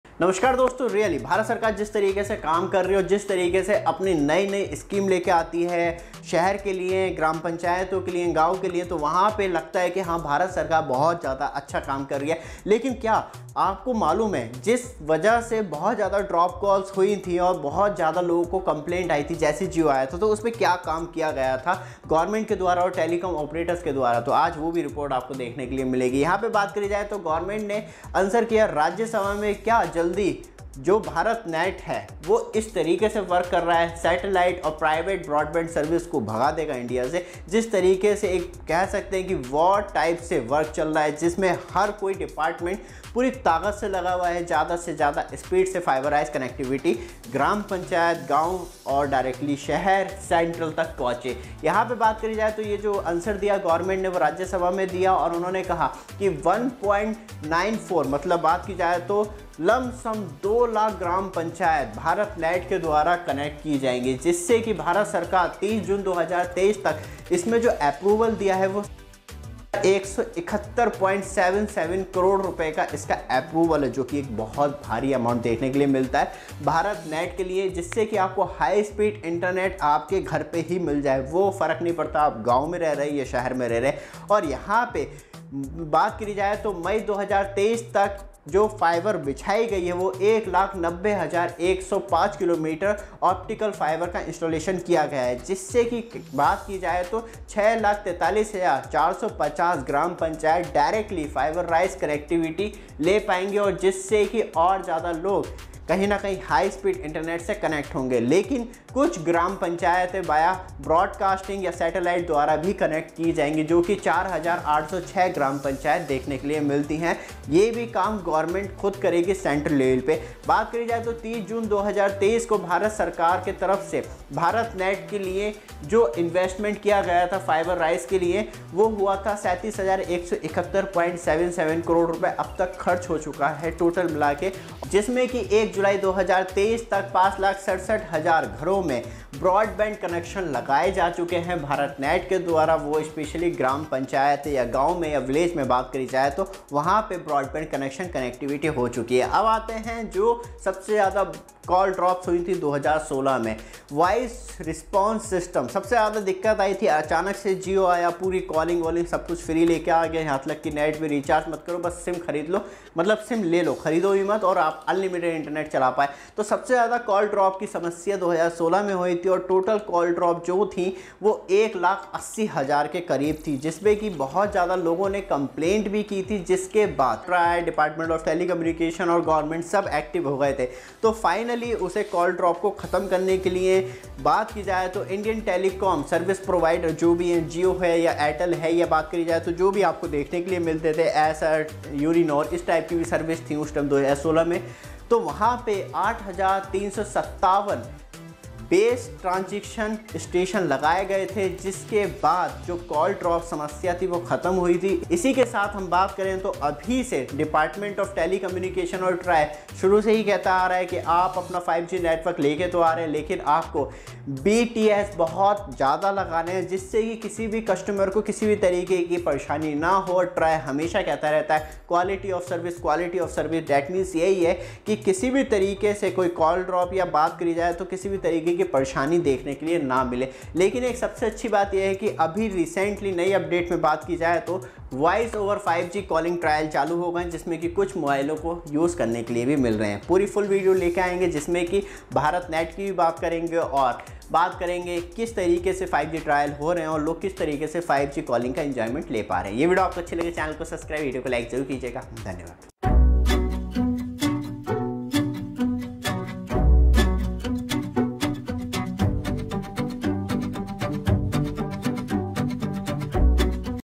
Bharat Sarkar Break Records on Fiber Broadband Connection on BharatNet Broadband Service in India
1.94 lakh gram panchayats service ready under BharatNet project: Chauhan Chauhan said that as of June 30, 2023, a sum of Rs 37,171.77 crore has been allocated under BharatNet project from Universal Service Obligation Fund (USOF). Telegram Facebook Copy Link ETTelecom Updated On Jul 26, 2023 at 05:31 PM IST NEW DELHI: A total of 1.94 lakh gram panchayats (GPs) have been made service ready under the BharatNet project in the country, the Parliament was informed on Wednesday. “As on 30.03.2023, 1,90,105 GPs have been made service ready on OFC media by laying 6, 43,450 Km Optical Fibre Cable (OFC). Further, 4,806 GPs have been made service ready on satellite media,” Minister of State (Mos) for Communications Devusinh Chauhan said in a written reply to an unstarred question in Lok Sabha. Chauhan said that as of June 30, 2023, a sum of Rs 37,171.77 crore has been allocated under BharatNet project from Universal Service Obligation Fund (USOF).